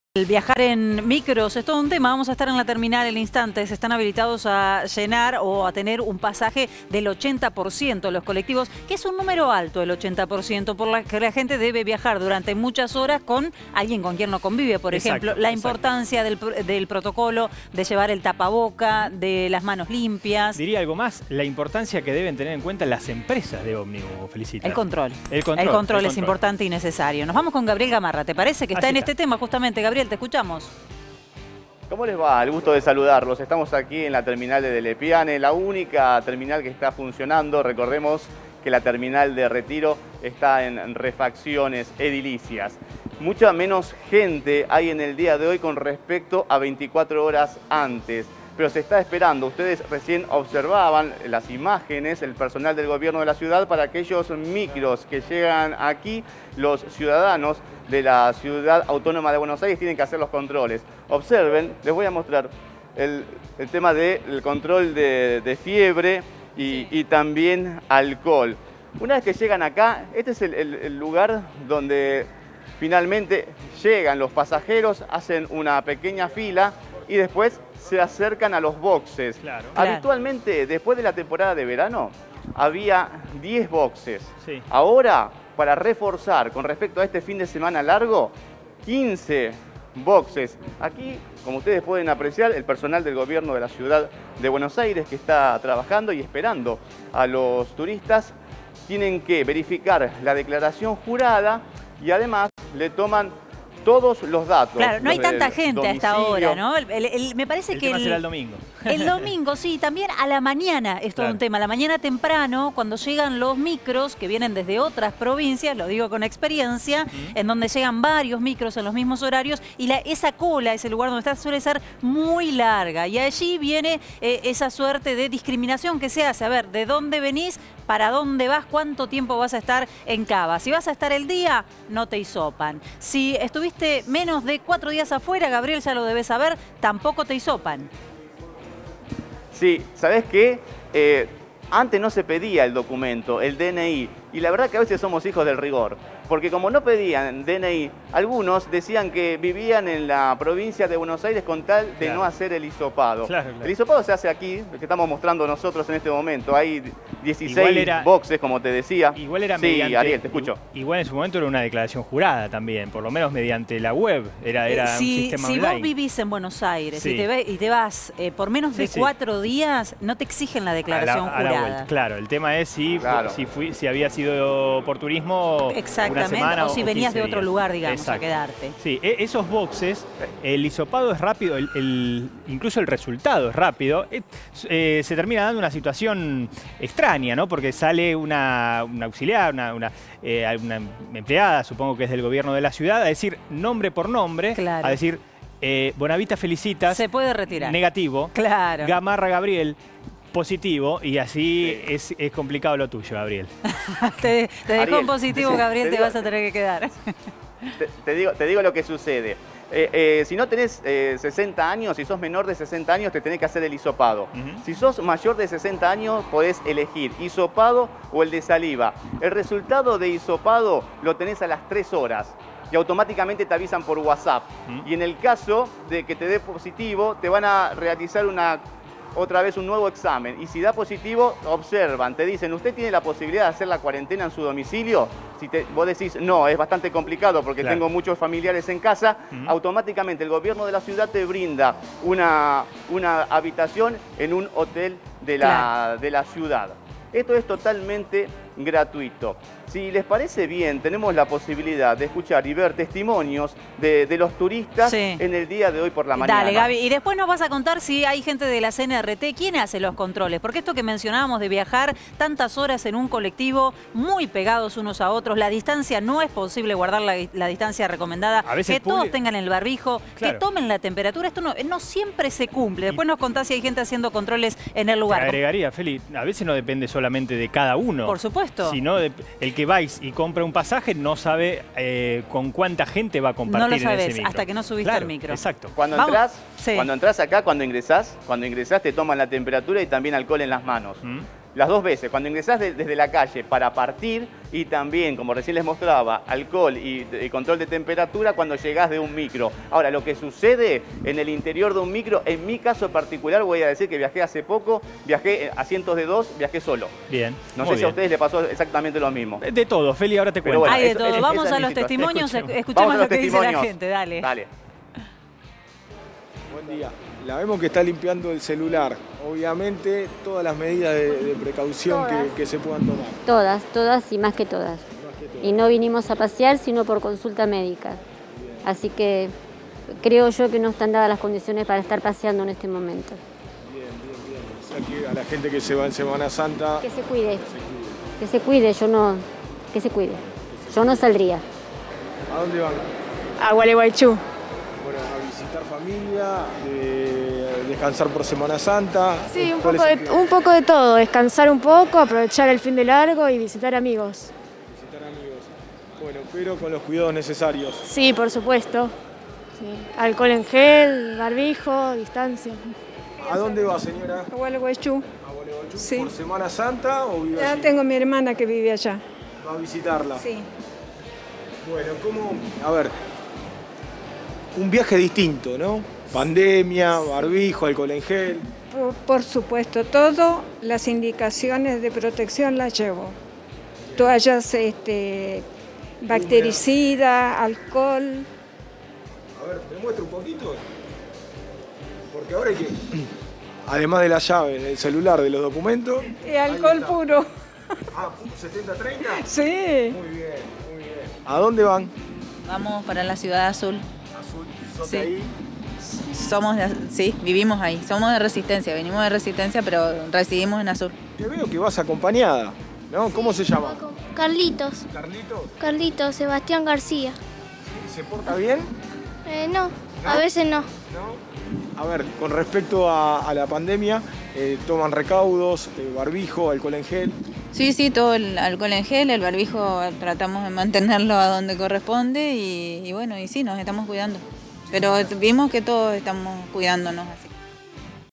Masivo movimiento turístico por Semana Santa
Las provincias refuerzan controles en el cumplimiento de protocolos sanitarios durante Semana Santa en los principales destinos turísticos del país, con intensificación de cuidados, operativos y campañas de concientización para evitar contagios de coronavirus.